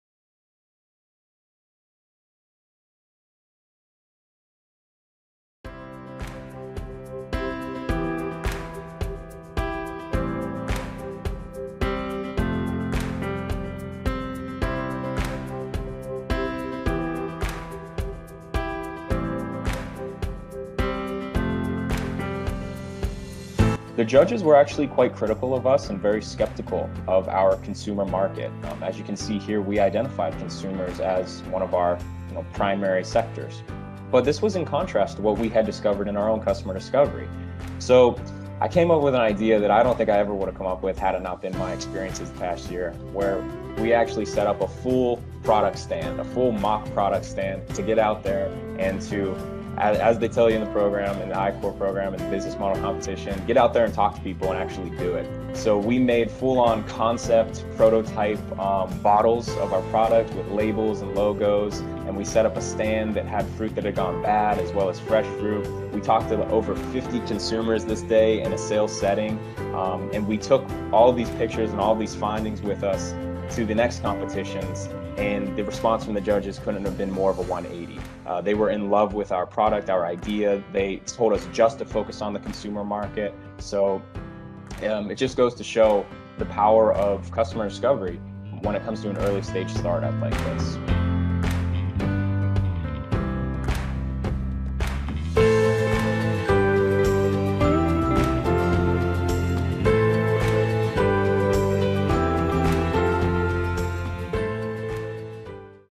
The Power of Customer Discovery
Graduate student, Alex Connor, explains his customer discovery process to validate one market vertical for his product.